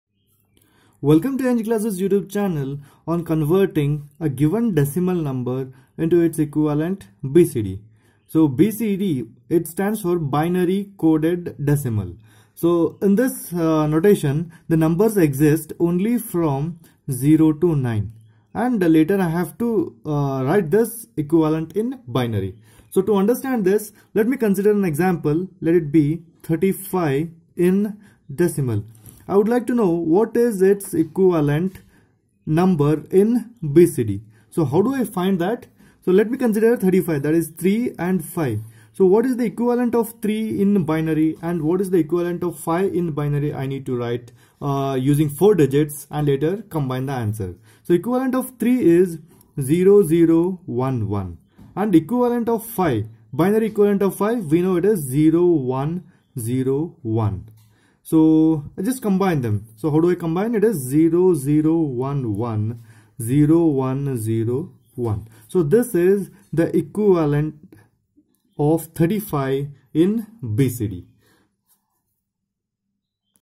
Decimal to BCD
Decimal to BCD conversion is explained in a very lucid way by considering an example in just 1 minute.